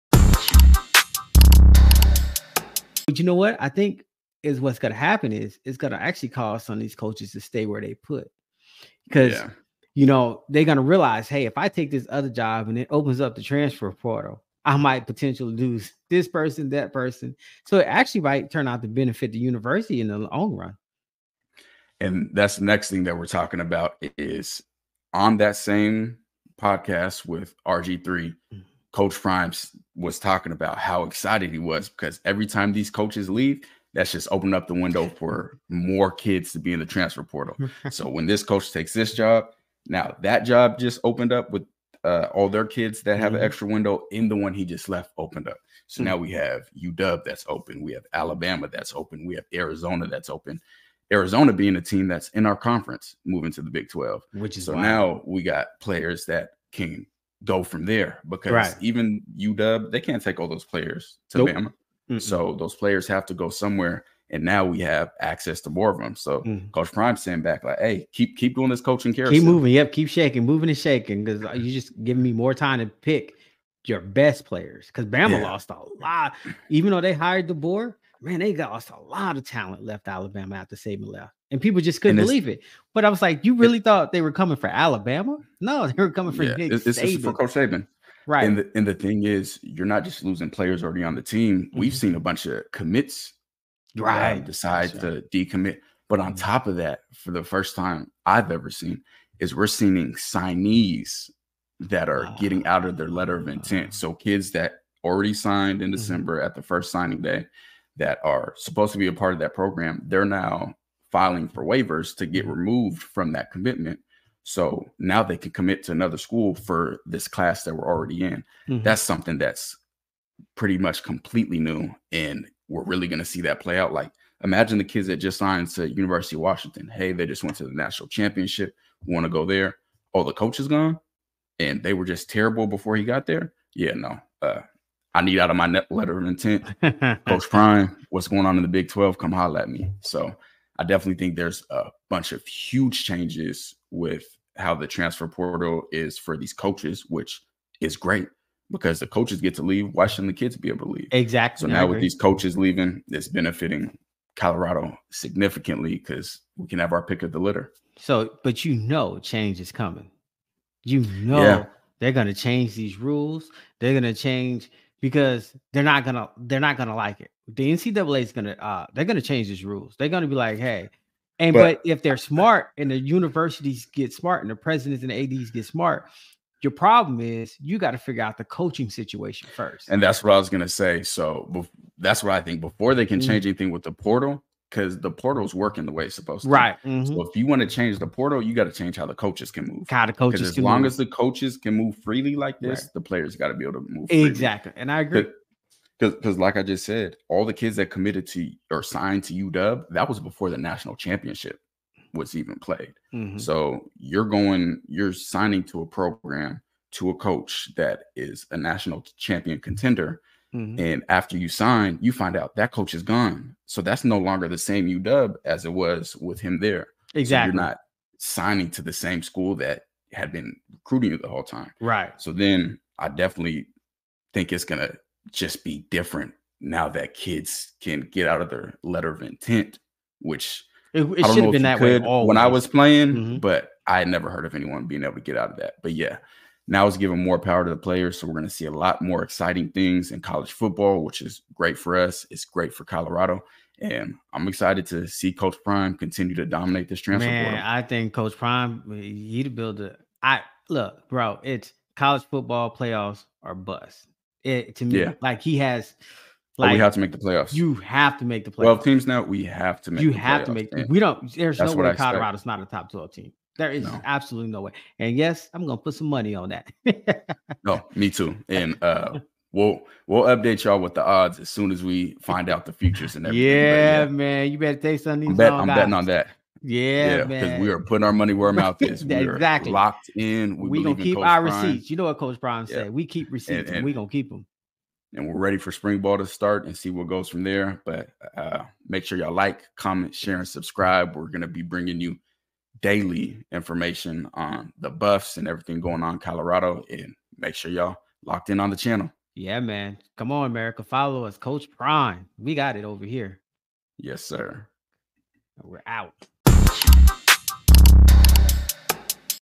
Coach Prime And Colorado Buffaloes Benefit From Coaches' Departures!
Coaches leaving their schools mean current players get a extra 30 day window to hit the transfer portal and ALSO allows for Signee's to get out of their NLI! We discuss the impact of the coaching carousel on the transfer portal and most importantly, coach prime and the colorado buffs!